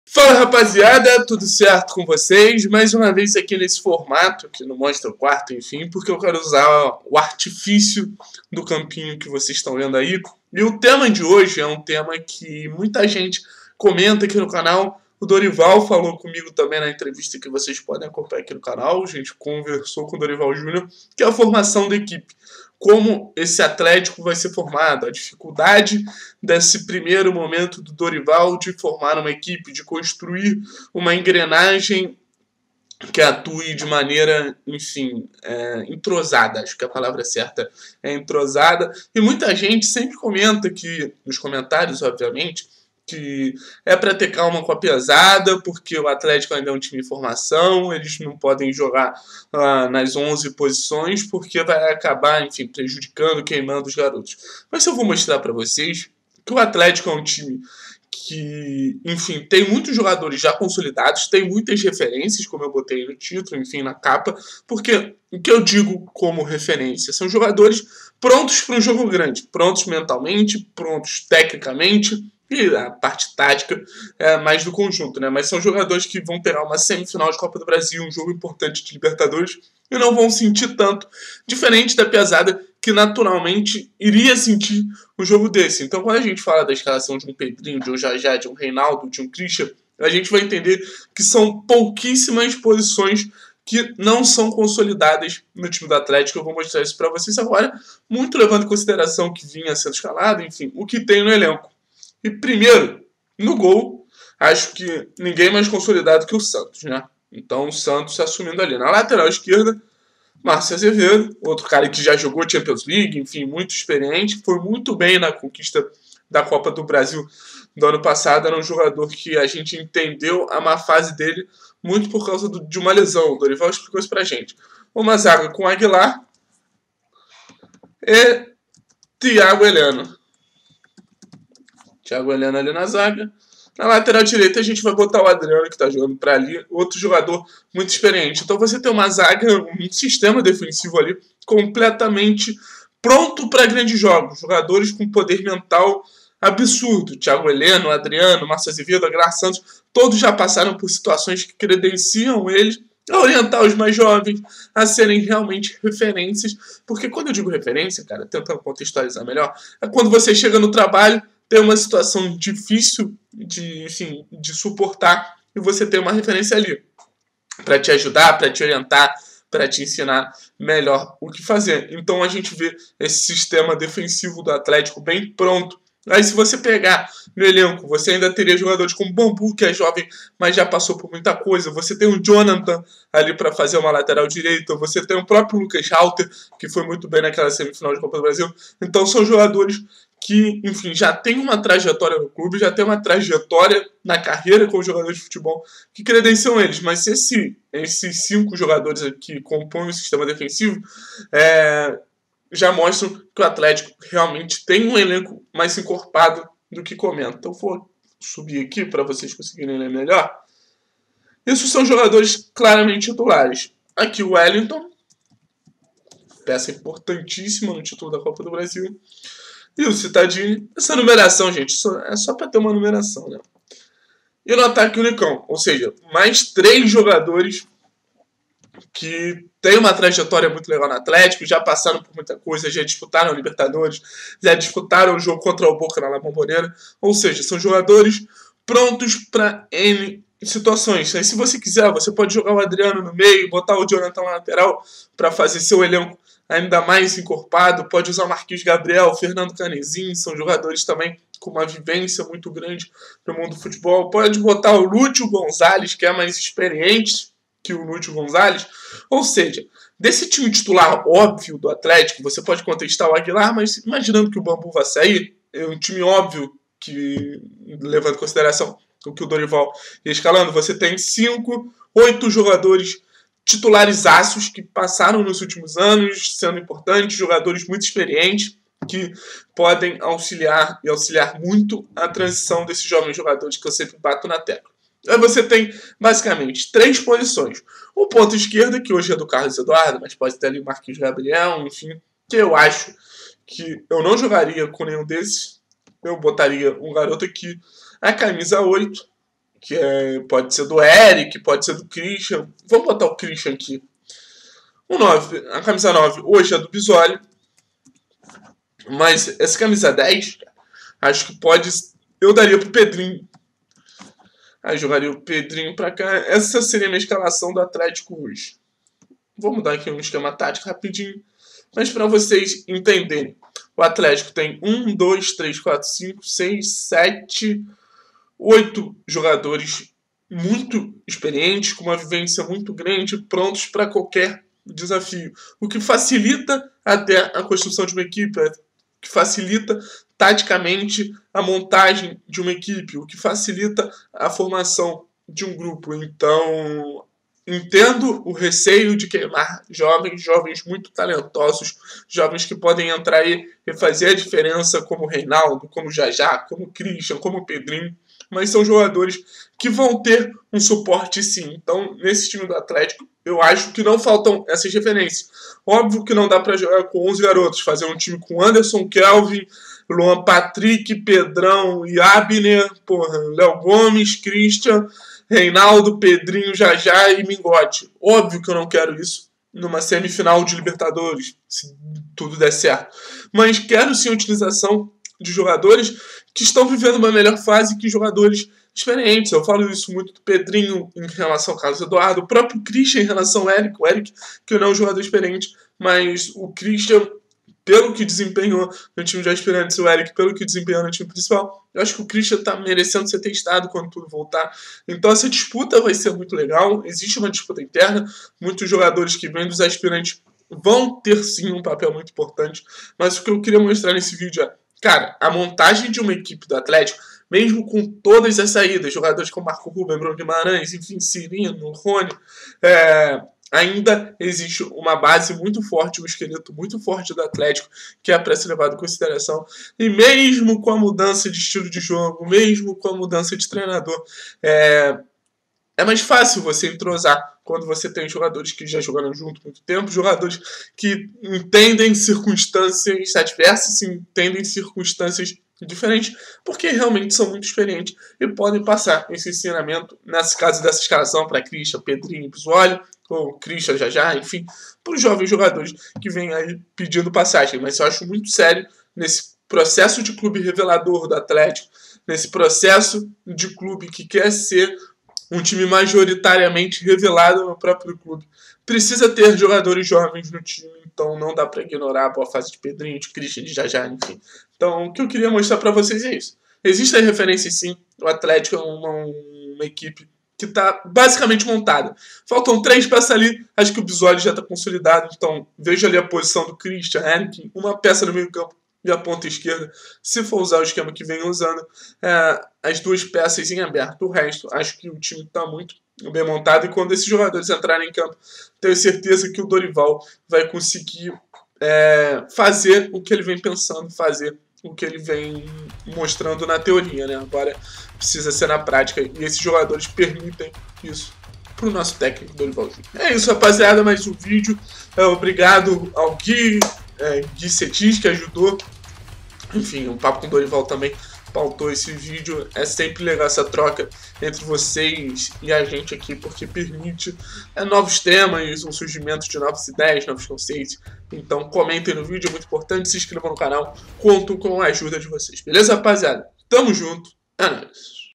FALTAM REFERÊNCIAS NO ELENCO DO ATHLETICO?
O elenco do Athletico é experiente o necessário? Faltam jogadores para receber os jovens que vão subir das categorias inferiores e vão auxiliar nos momentos mais complicados da temporada?

Eu acho que sim, vou tentar te convencer!